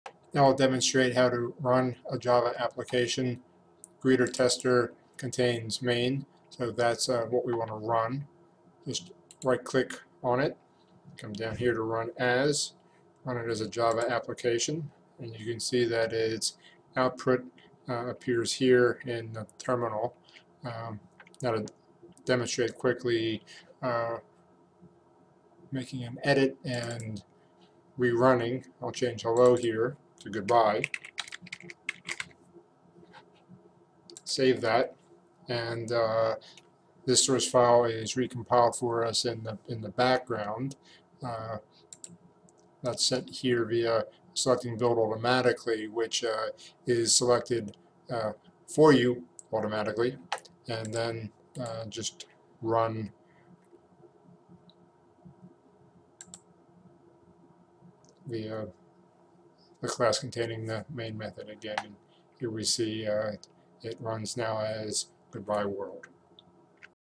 Eclipse Run, Edit, Save, and Run Cycle
This how-to demonstrates the run, edit, save, run cycle for a Java project in Eclipse.